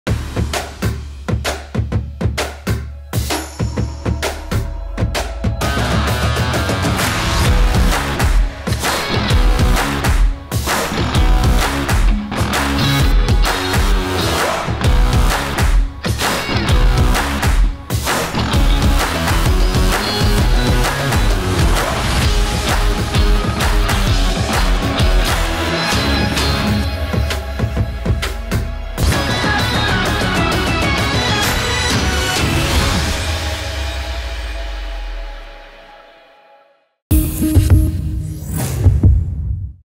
Re-engineered For You | GCC Hot Weather Testing - Short Version
This generation of Peugeot  is different. Find out how we’ve re-engineered our cars to beat the desert heat.